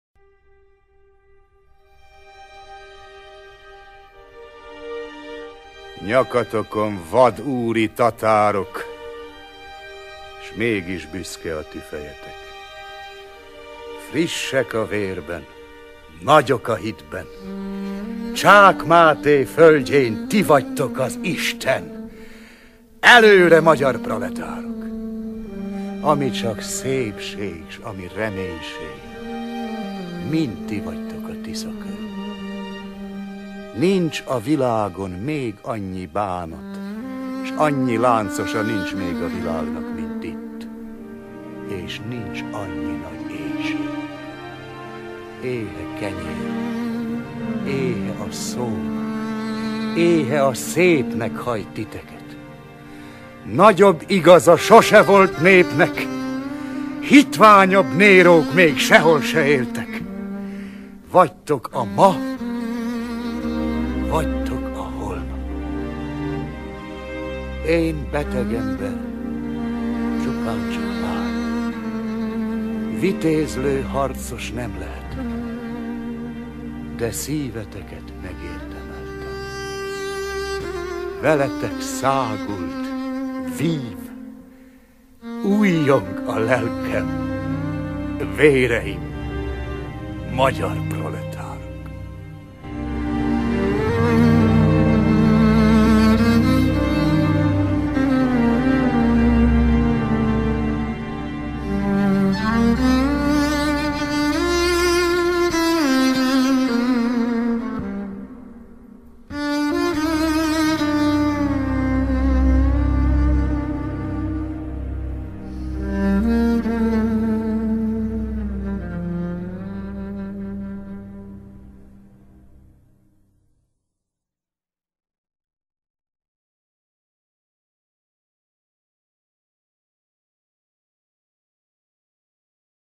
Ady Endre: Csák Máté földjén (Latinovits) zenével
Nyakatokon vad, úri tatárok
S mégis büszke a ti fejetek.
Frissek a vérben, nagyok a hitben,
Csák Máté földjén ti vagytok az Isten.
Elõre, magyar proletárok.

Ami csak szépség s ami reménység,
Mind ti vagytok a Tisza körül.
Nincs a világon még annyi bánat
S annyi láncosa nincs még a világnak,
Mint itt és nincs annyi nagy éhség.

Éhe kenyérnek, éhe a Szónak,
Éhe a Szépnek hajt titeket.
Nagyobb igaza sohse volt népnek,
Hitványabb Nérók még seholse éltek.
Vagytok: a Ma, vagytok: a Holnap.

Én, beteg ember, csupáncsak várok,
Vitézlõ harcos nem lehetek.
De szíveteket megérdemeltem,
Veletek száguld, vív, ujjong a lelkem:
Véreim, magyar proletárok.

1907.